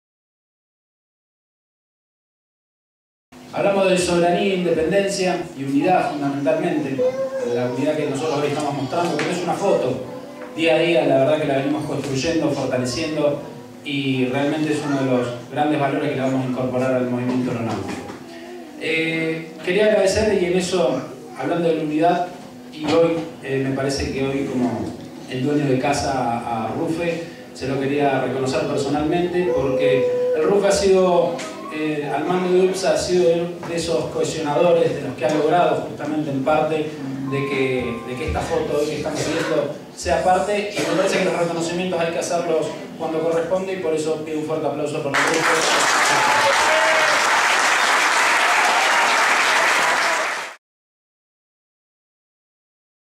Genaro Trucco, Día de la indep Cielos Soberanos
FRENTE GREMIAL AERONAUTICO día de la Independencia, Secretario General de UALA Unión Aviadores de Líneas Aéreas.